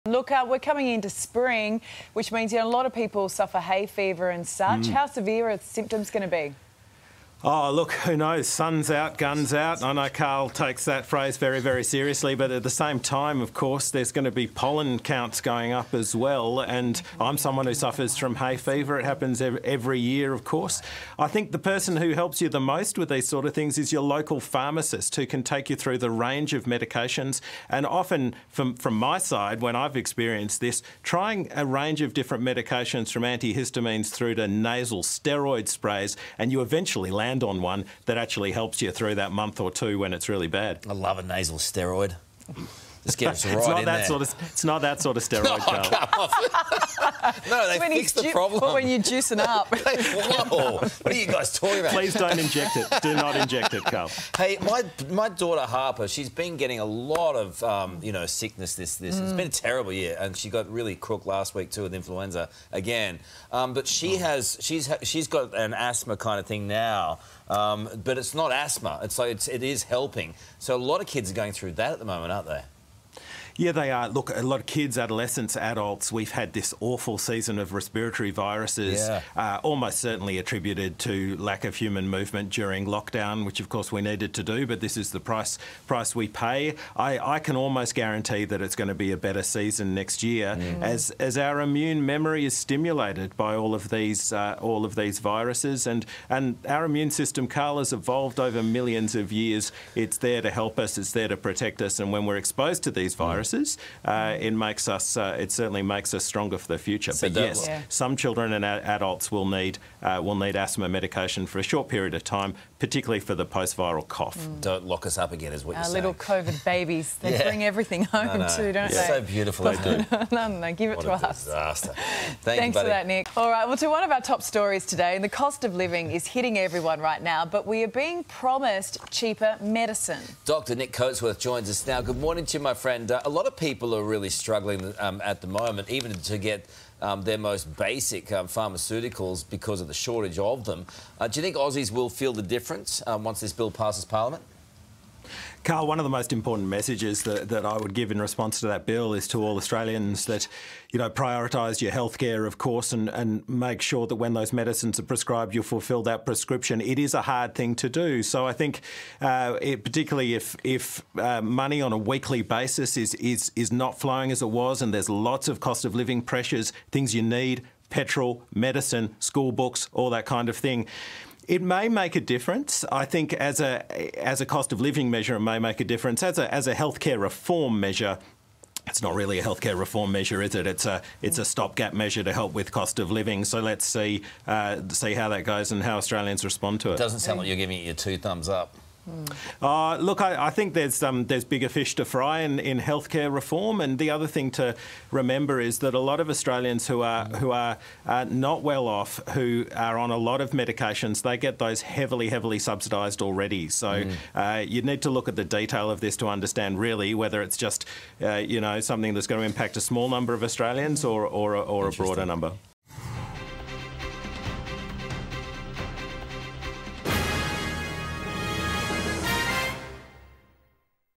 How to fight hay fever this spring season | 9 News Australia
Today Health expert, Dr Nick Coatsworth, shares some ways for the millions of Australians who suffer from allergies to combat hay fever this spring.